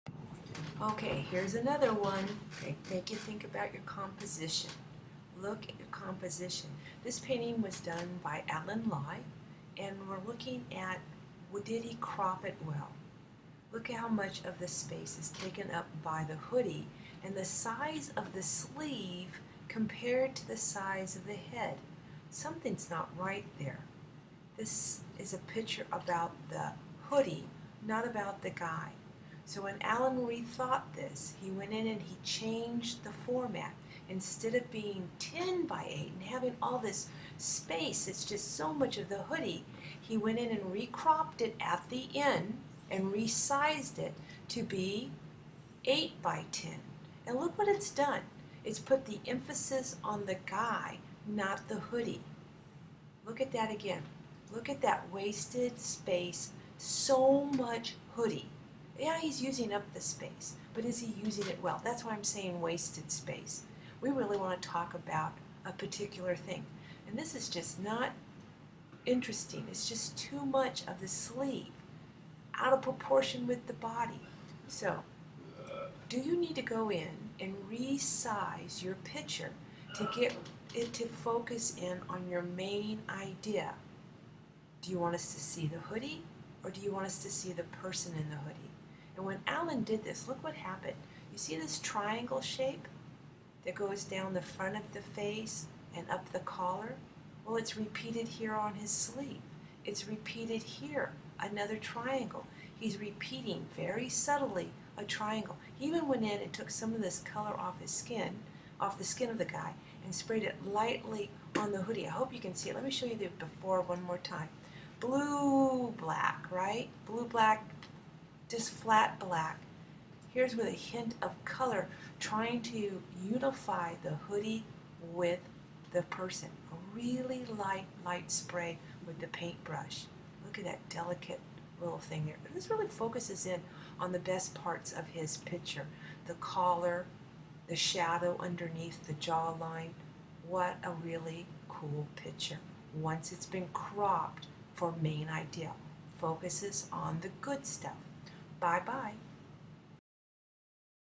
Hoodie #7: Cropping For Meaning
OK, so you think your done.  Take some time to see what Allen Li did to improve his finished painting with cropping again for meaning.